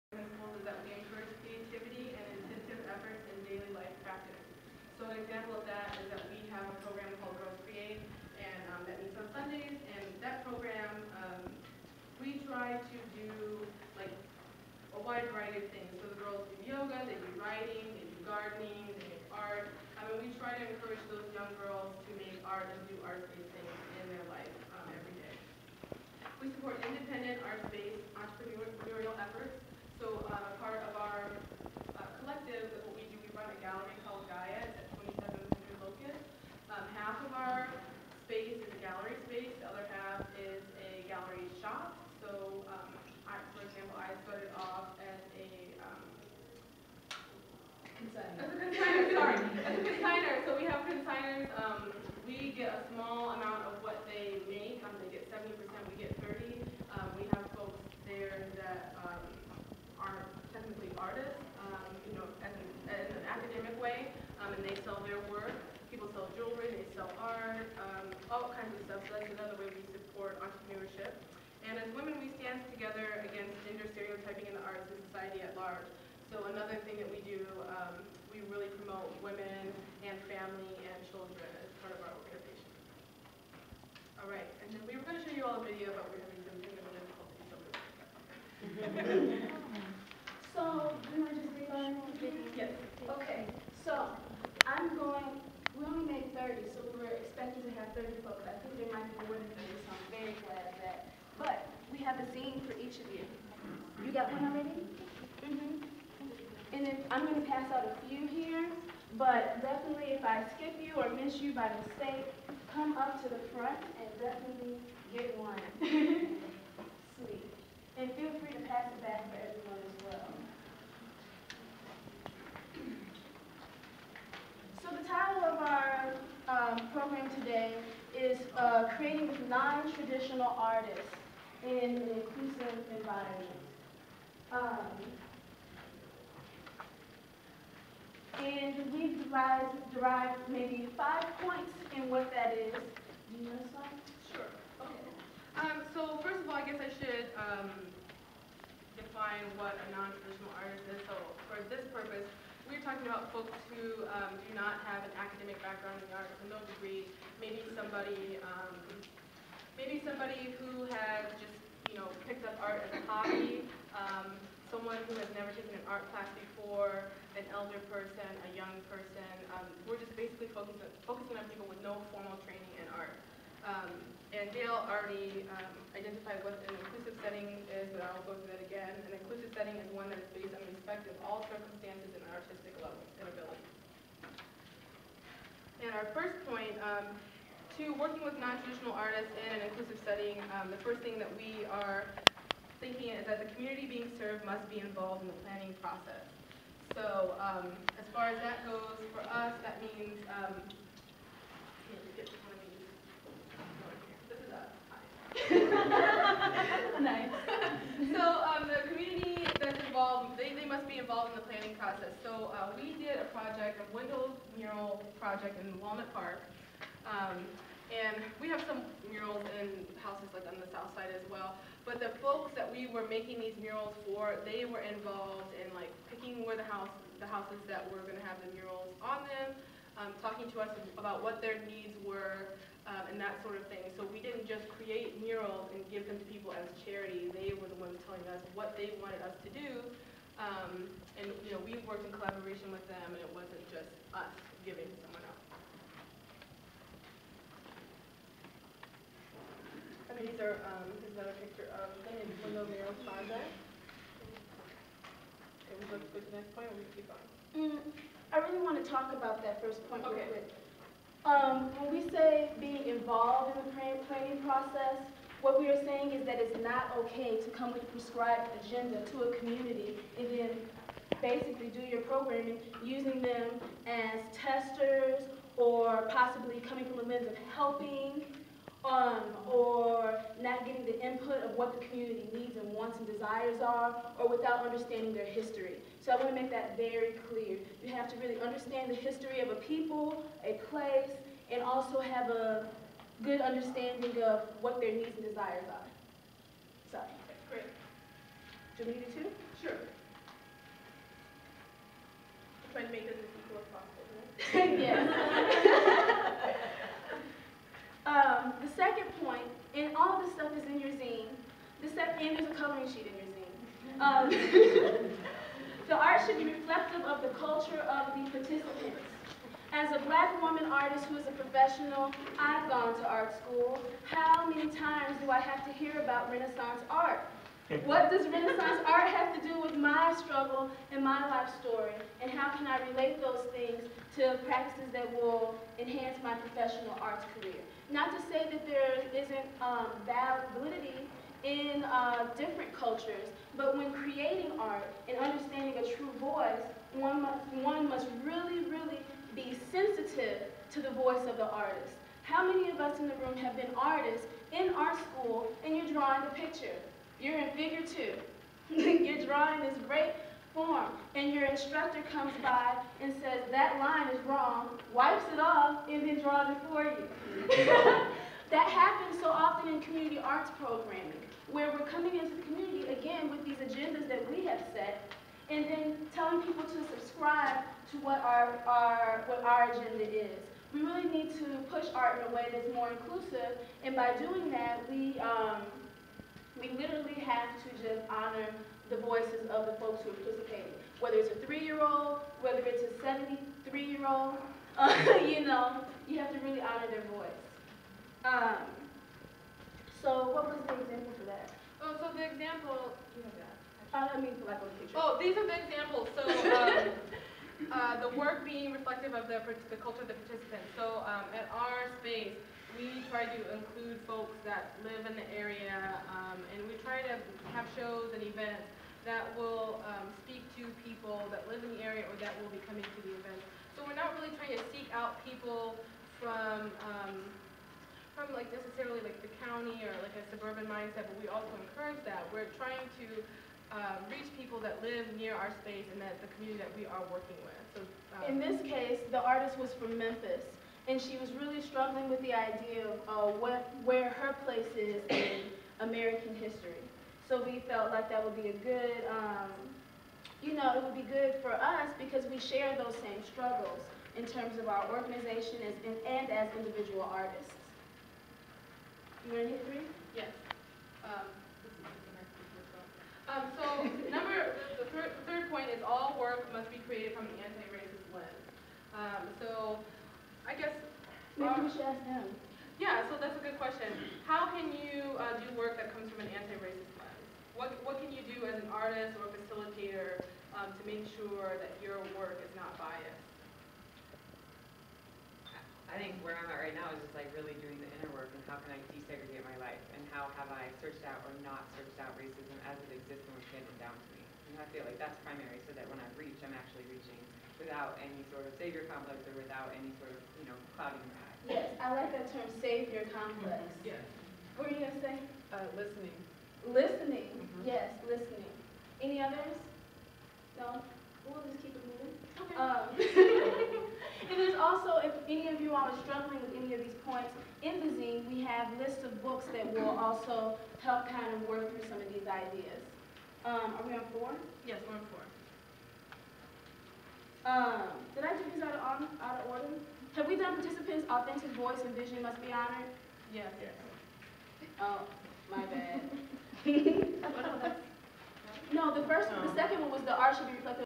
Creating With Nontraditional Artists 1
This workshop was facilitated by two members of Yeyo Arts Collective during the Artistbelt to Rustbelt Conference held in Saint Louis, Mo.